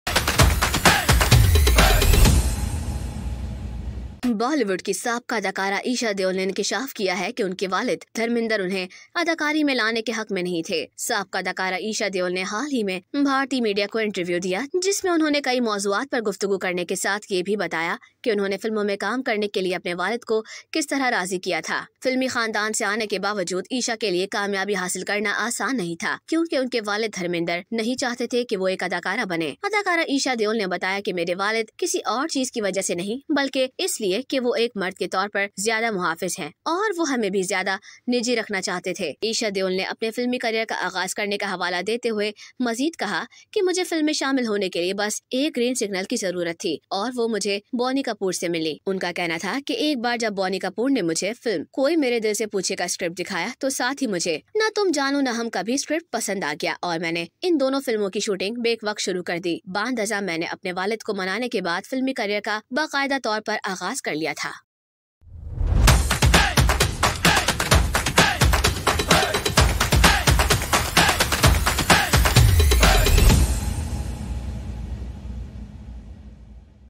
Esha Deol Reveals, Father Dharmendra Didn't Want Me To Act | Hungama Express
Esha Deol Reveals, Father Dharmendra Didn't Want Me To Act